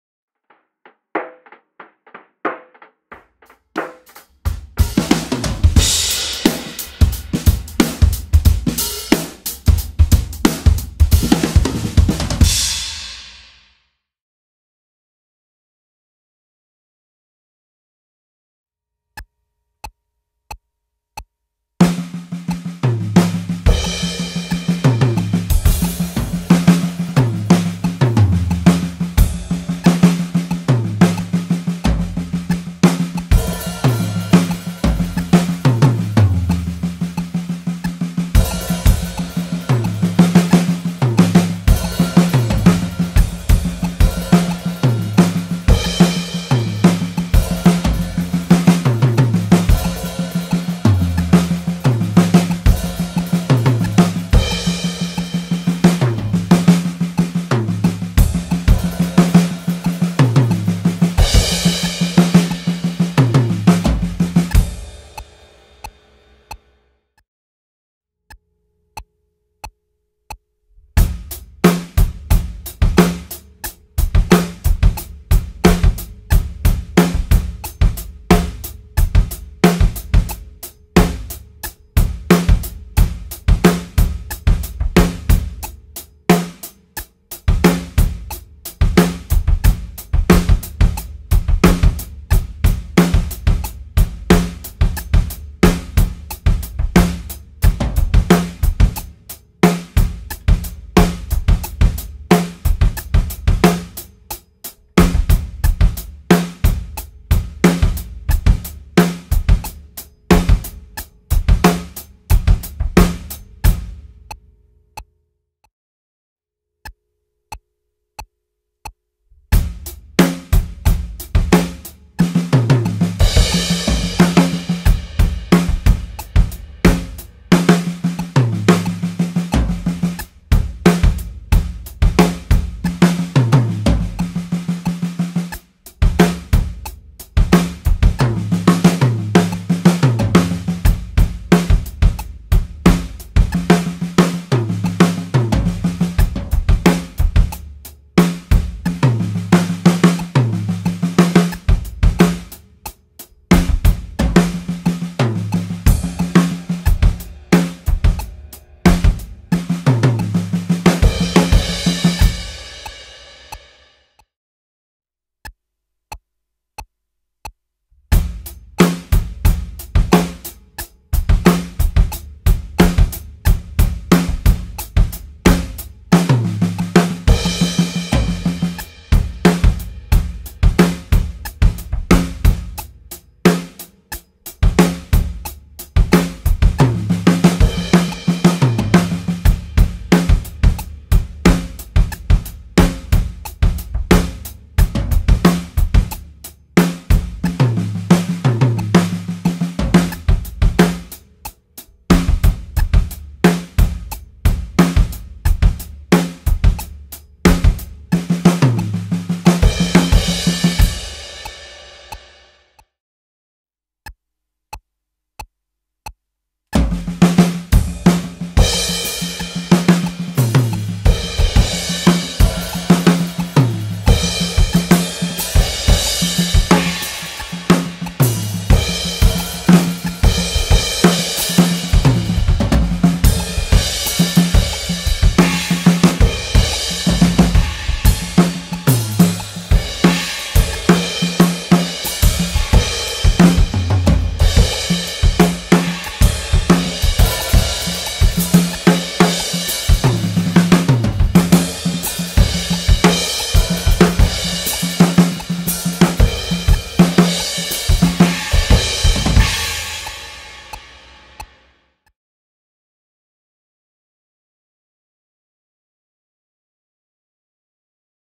5 Ways To Interpret A Drum Chart - Free Pdf Lesson & Demo
🚀Make 2024 Your Best Drumming Year!
Simply put, reading music is not about reading but is about interpretation. That’s why being a good reader is not about literal execution of what’s written. It’s about adding what’s not written, which is most of what we actually play. Here we are going to use a 16th note drum chart to learn 5 different approaches, which are very helpful in becoming more comfortable and confident about our reading skills.

Intro 0:00
Phrasing 0:18
Groove 1:07
Song Form 1+1 1:56
Song Form 3+1 2:44
Free Interpretation: 3:33